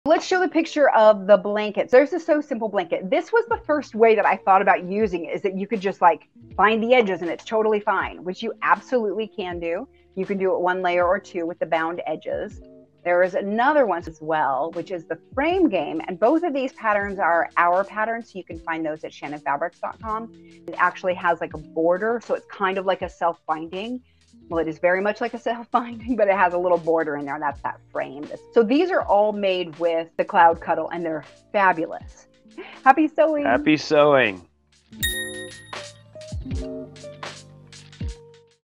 What to Use our New Cloud Cuddle® For? 💡☁️These Blankets are Perfect
There's so much buzz around our new Cloud Cuddle®. What is it and what do you use it for?

This exclusive Cuddle® minky plush fabric is double-sided—with a Digital Cuddle® print on one side (3mm) and a plain white backing (4mm) on the other side. Latte Cloud Cuddle® features an aerial view of latte art in coffee mugs overlaid on a solid background. This fabric is perfect for beginner sewists because it does not require a backing for the finished product, making it easier than ever to sew blankets, scarves, and beyond!

What to use it for? This double-sided fabric lends itself to a wide array of projects. The versatility of Cloud Cuddle® makes it a great choice for throw blankets, quilts and pillows as well as kids' apparel like comfy loungewear and pajamas as well as bathing suit cover-ups and so much more.

Here, Teresa mentions 2 of our popular free patterns, the Sew Simple blanket and the Frame Game

Fabrics featured:
Sunflower Self-Binding Blanket
Pattern: Sew Simple Blanket
Fabrics: CCSunflower Sunshine, LC Glacier Basil

Latte Blanket
Pattern: Frame Game
Fabrics: CCLatte Aqua Sea, LC Froosted Hide Camel, C3 Camel